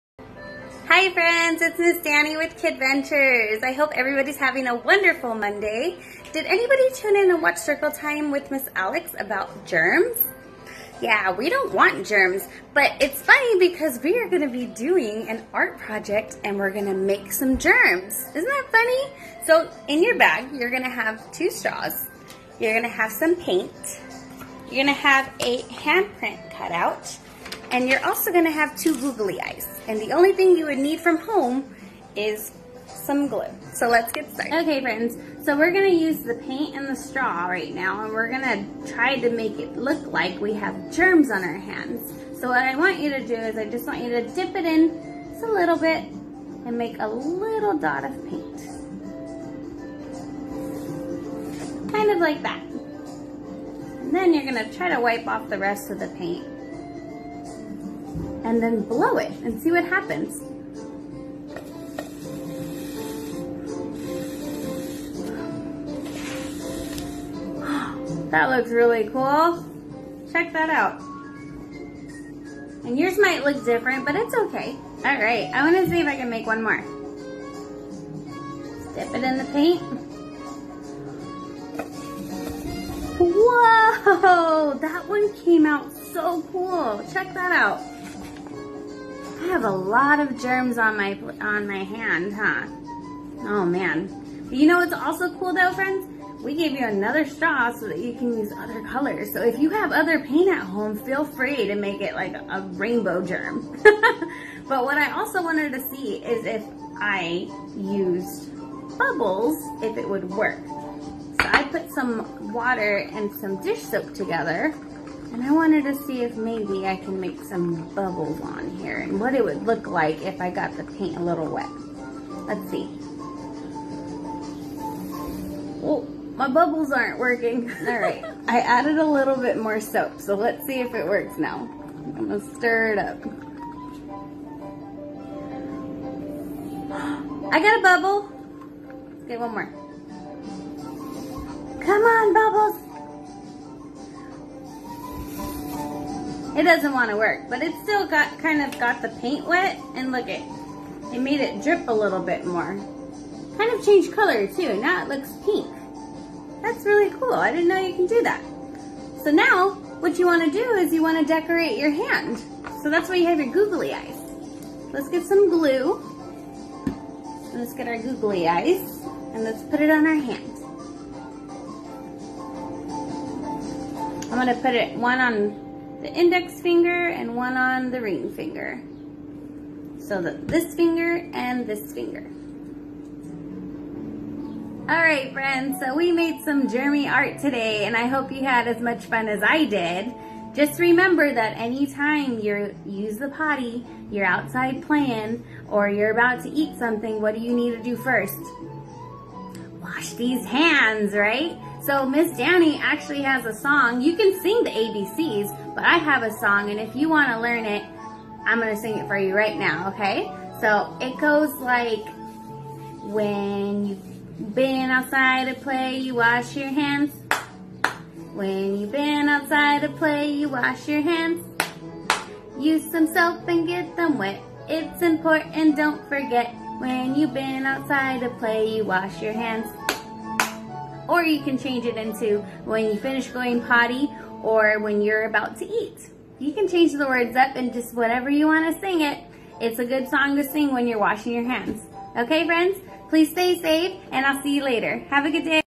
Blow Germ Art Project with Ms. Danni
Come paint with Ms. Danni and watch as she recreates germs!  See how they spread with just air?  How big can you make your germs?